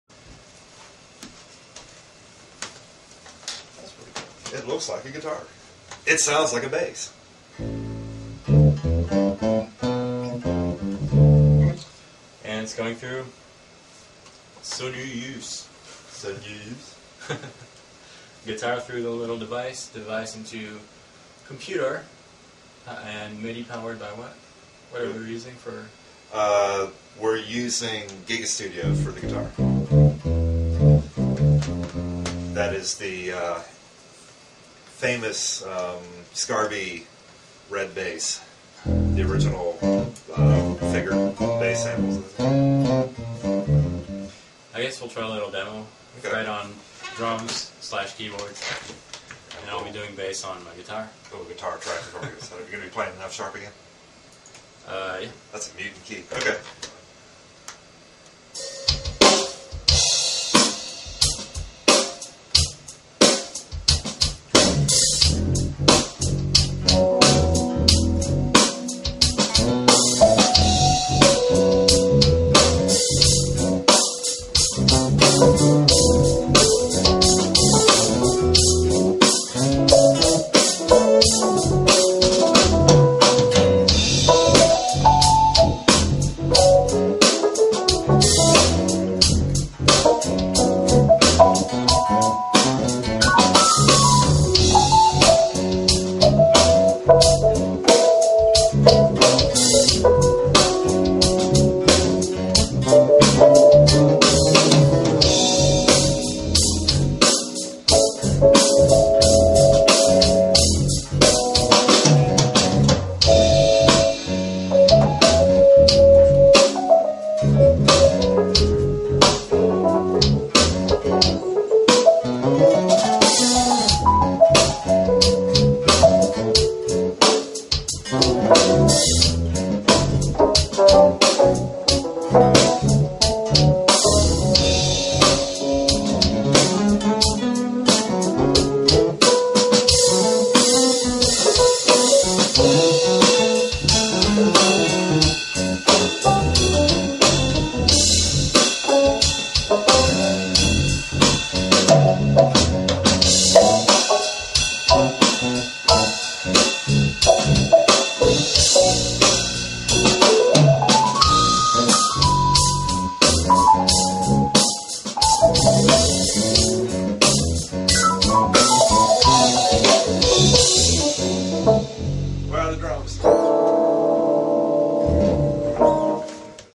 Sonuus G2M demo
Here's a little demo of the G2M Guitar to Midi converter from Sonuus.
This little jam shows how usable the device is for live applications. Fred Shendel on keyboards and myself on guitar. Hope you enjoy this!